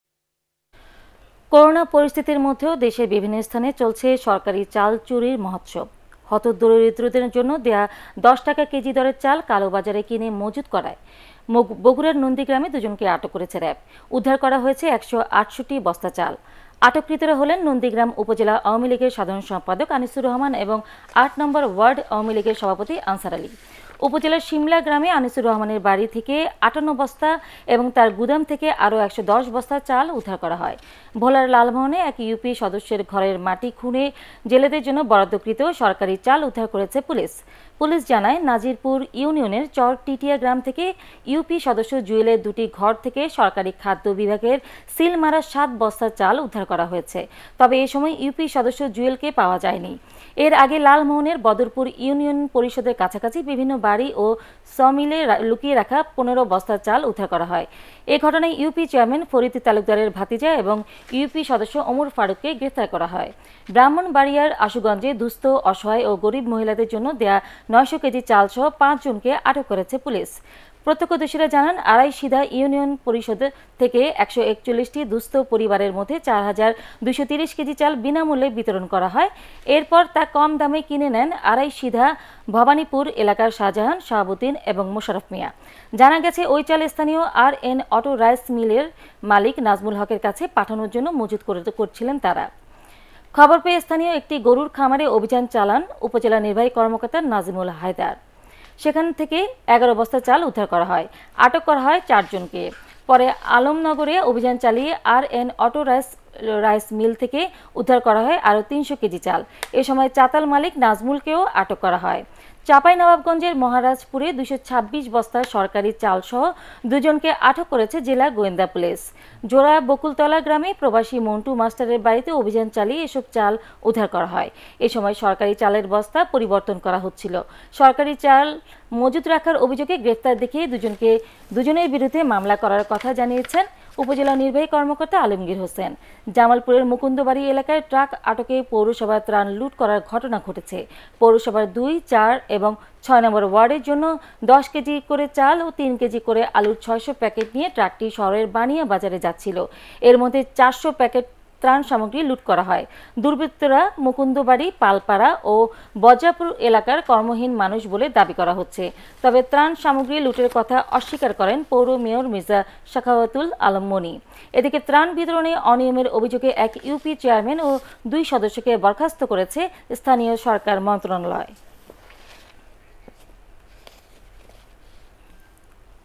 চাউল চোর সামাচার । করোনায় চাউল চোর । ATN Bangla News
করোনা পরিস্থিতির মধ্যেও দেশের বিভিন্ন স্থানে চলছে সরকারি চাল চুরির মহোৎসব। হতদরিদ্রদের জন্য দেয়া ১০ টাকা কেজি দরের চাল কালোবাজারে কিনে মজুদ করায় বগুড়ার নন্দীগ্রামে দু’জনকে আটক করেছে র‌্যাব। উদ্ধার করা হয়েছে ১৬৮ বস্তা চাল। আটককৃতরা হলেন: নন্দীগ্রাম উপজেলা আওয়ামী লীগের সাধারণ সম্পাদক আনিসুর রহমান এবং আট নম্বর ওয়ার্ড আওয়ামী লীগের সভাপতি আনসার আলী। উপজেলার সিমলা গ্রামে আনিসুর রহমানের বাড়ি থেকে ৫৮ বস্তা এবং তাঁর গুদাম থেকে আরো ১১০ বস্তা চাল উদ্ধার করা হয়।